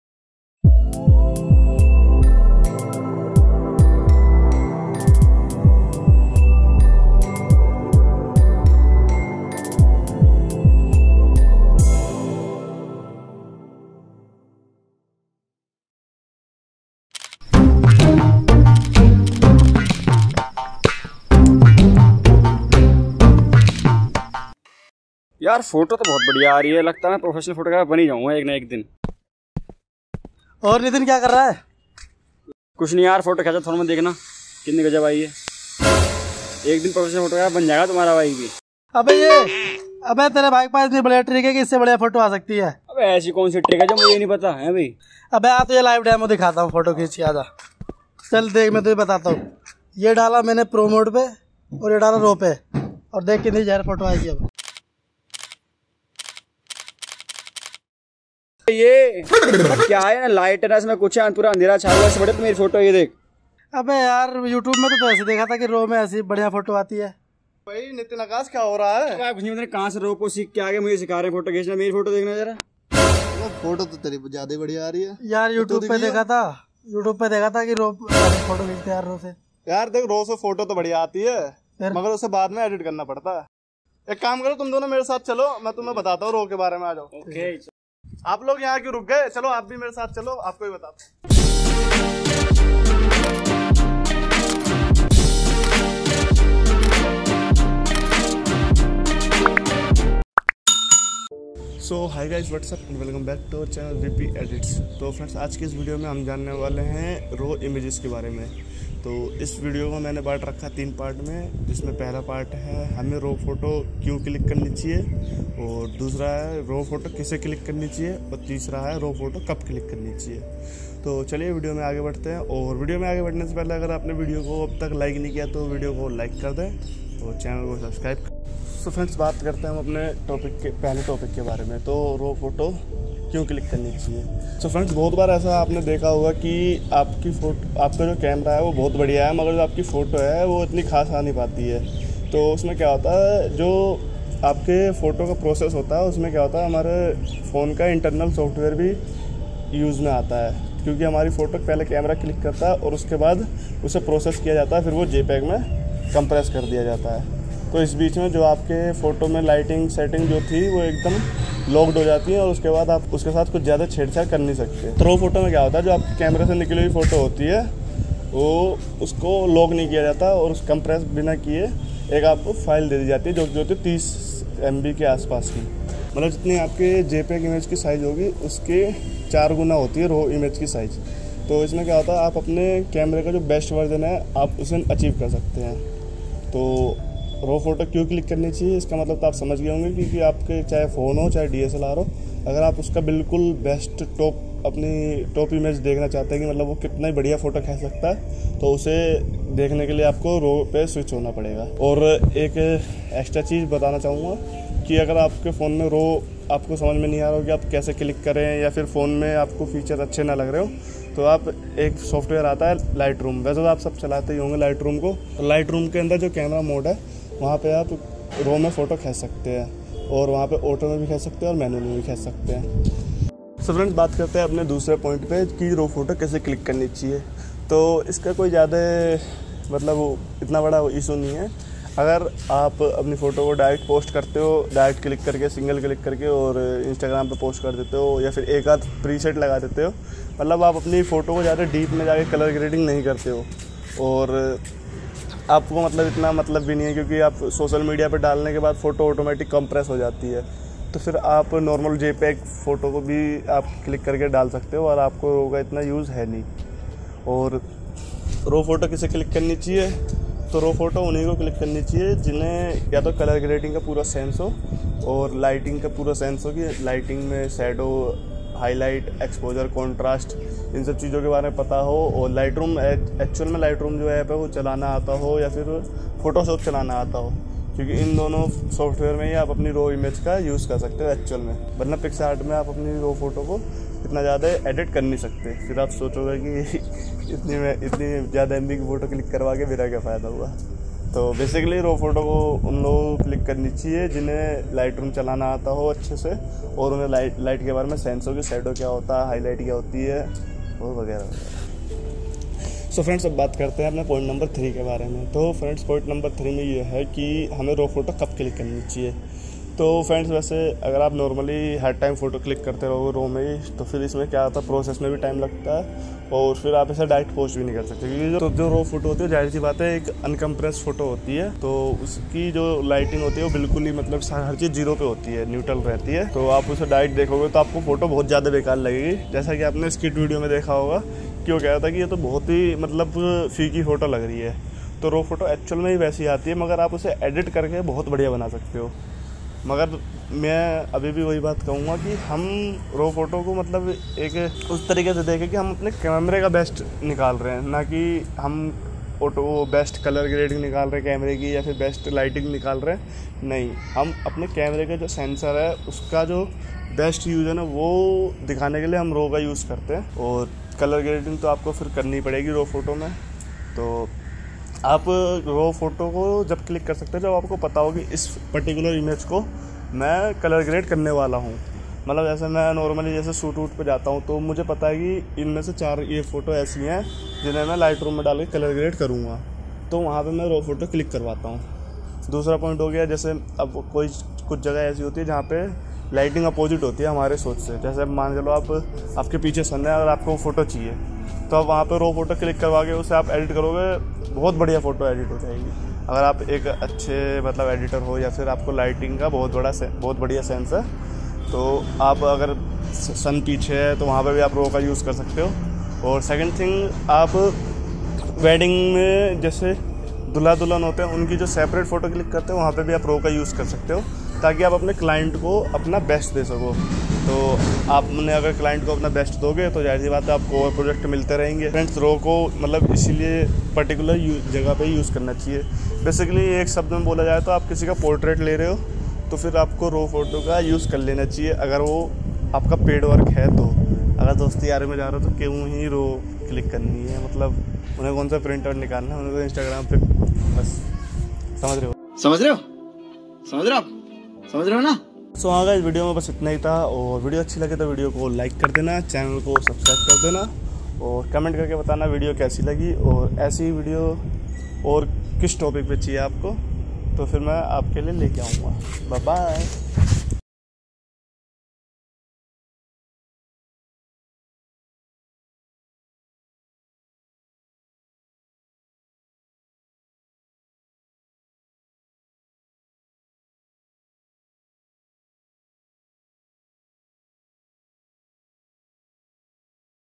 RAW IMAGES
RAW IMAGES || WHAT IS RAW PHOTOS || VPEDITS     @NSB Pictures

IN THIS VIDEO I TRYING TO EXPLAIN ABOUT RAW IMAGES AND DIFFERENCE BETWEEN  RAW AND JPEG
VIDEO ACHI LAGE TO VIDEO KO LIKE KARE
KOI SUJHAB HO TO COMMENT KAR K BATAYE

equipment i use
lens 1
lens 2
 . Social media.